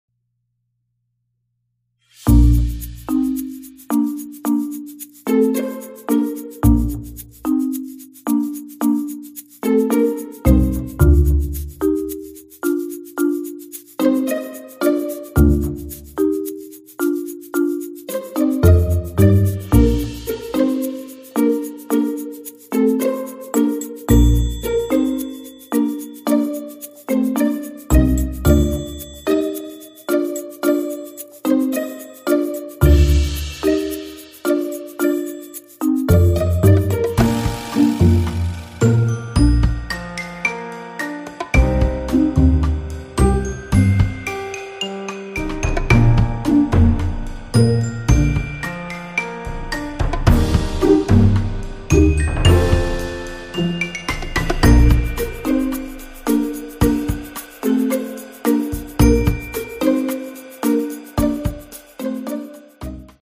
Faire du vélo à Orléans Métropole - sous les flocons dans la neige fraîche
Modérer l'allure, mouliner davantage et profiter du moment. :-)